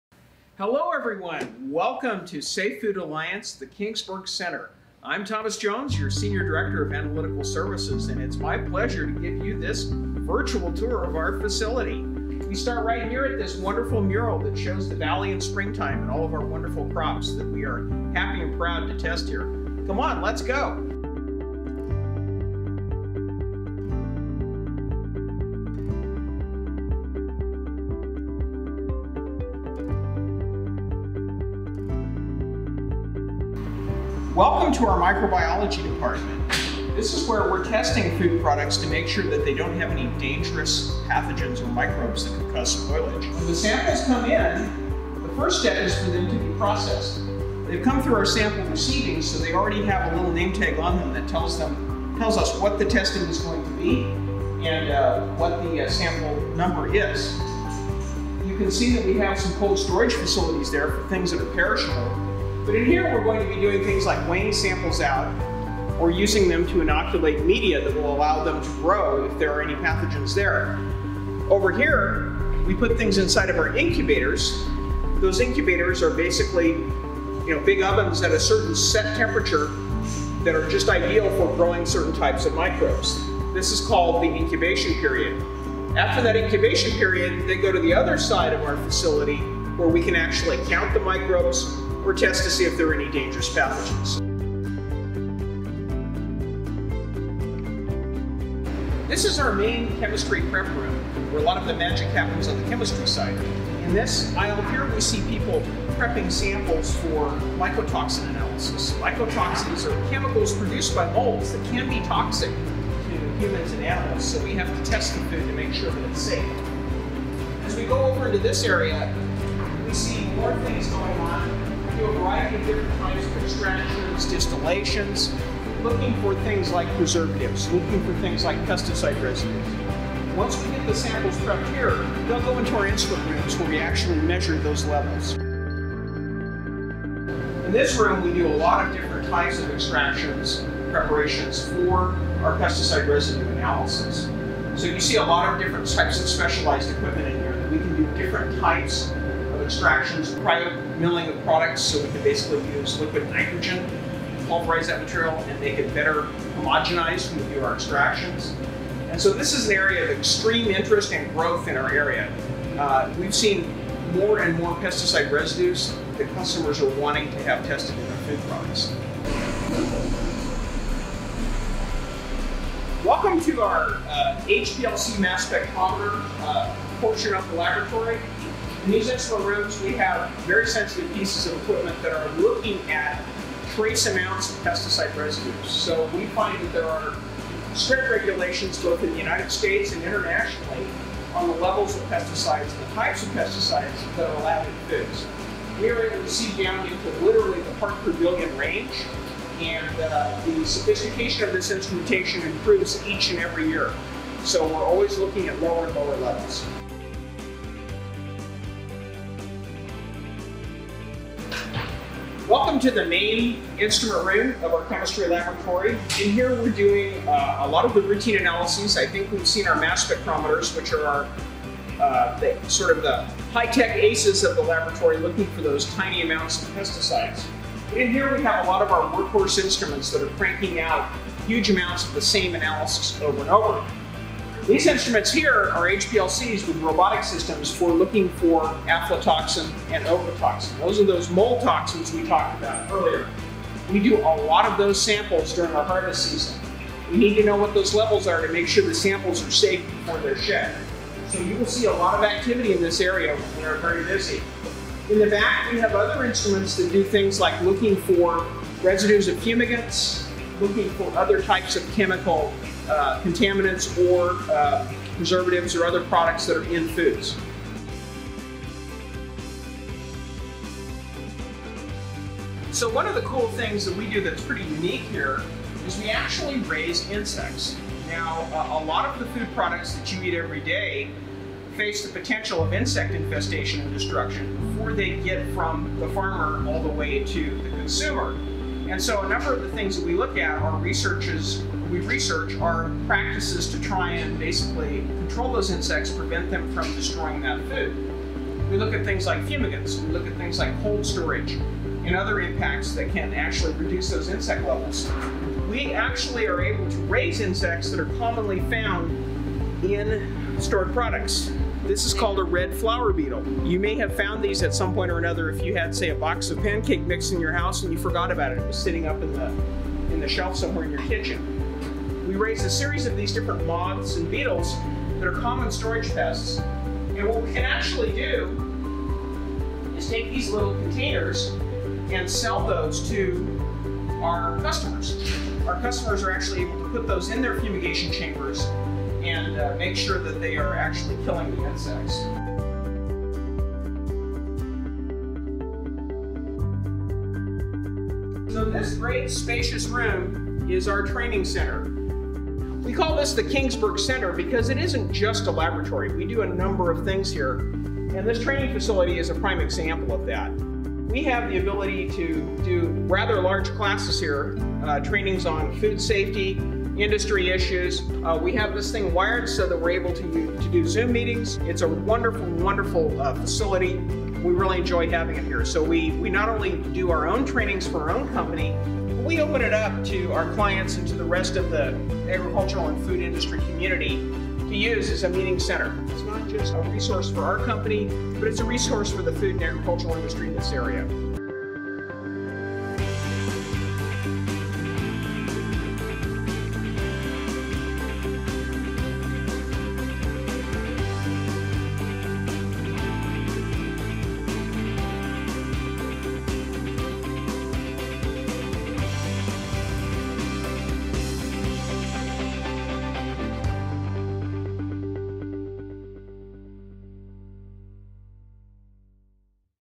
Inside a Food Laboratory - Virtual Tour
Take a virtual journey through our cutting-edge Kingsburg Food Safety Laboratory with Thomas Jones, our Senior Director of Analytical Services!

Step inside the Central Valley's premier food safety facility and discover the advanced technologies and innovative processes that power our operations. From state-of-the-art equipment to rigorous quality control measures, Thomas provides an exclusive behind-the-scenes look at how we ensure the safety and integrity of food products.

Join us as we explore:
- High-tech laboratory instrumentation and capabilities
- Specialized testing procedures for detecting contaminants and ensuring product quality
- Quality assurance protocols and regulatory compliance measures

Whether you're a food industry professional, a regulatory official, or simply curious about the science of food safety, this virtual tour offers invaluable insights into our commitment to excellence.

to learn more about our services and capabilities.